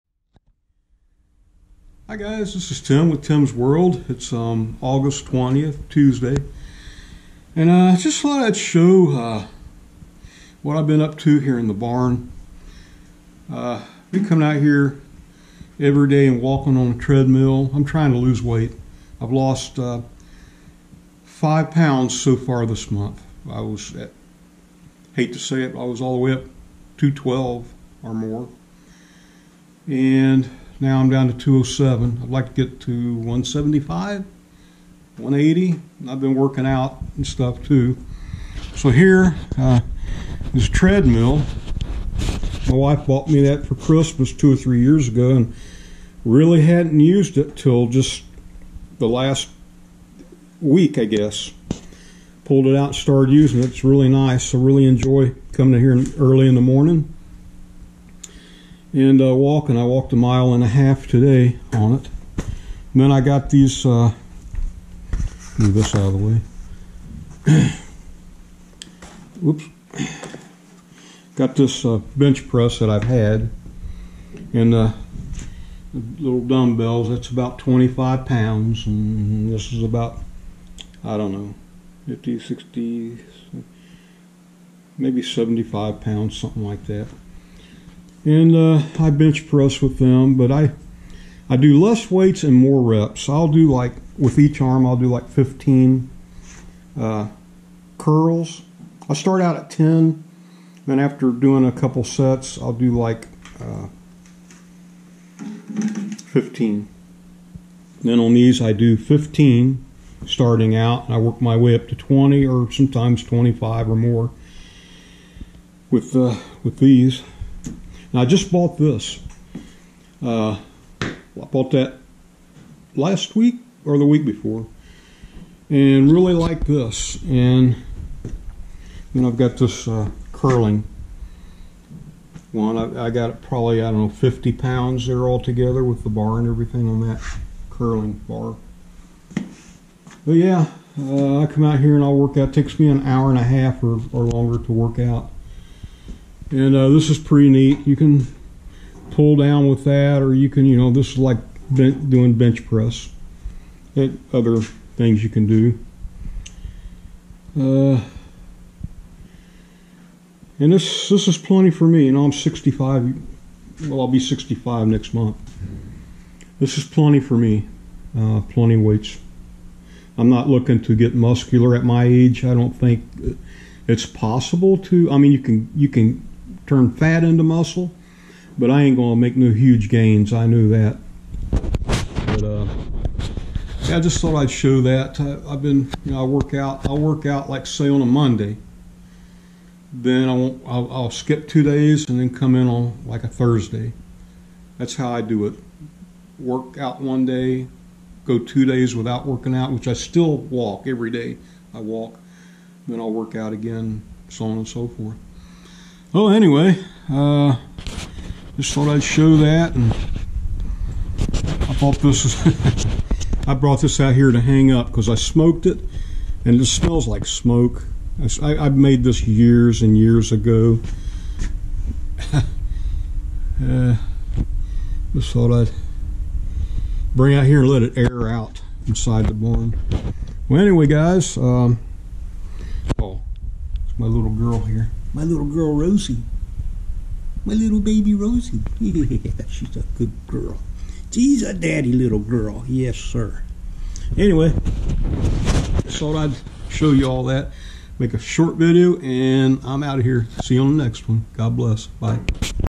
my obesity battle trying to lose weight
trying to lose this big belly is a battle,but i am determined to win.